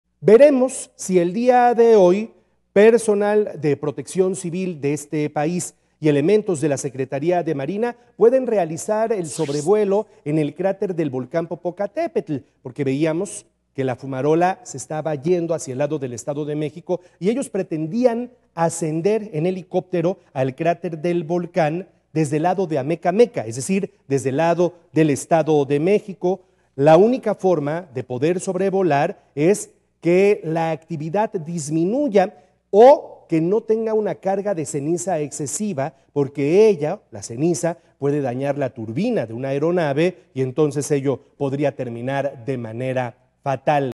Sobrevuelo volcán || Noticias con Juan Carlos Valerio
Hoy podría realizarse un sobrevuelo sobre el cráter del volcán Popocatépetl.

Aquí detalles.